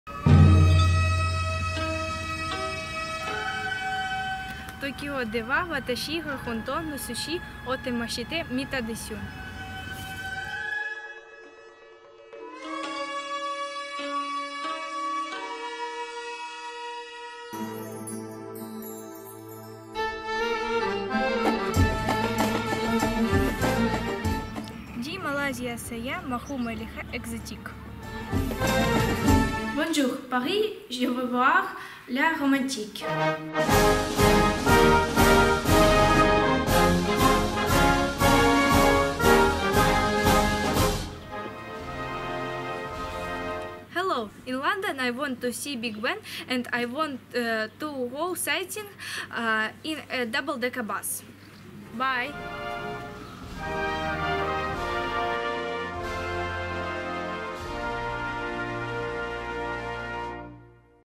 My dream by Ulyana Chernikh, Ukraine 2017
The One Minutes Jr. workshop in Severodonestk, Ukraine in collaboration with UNICEF.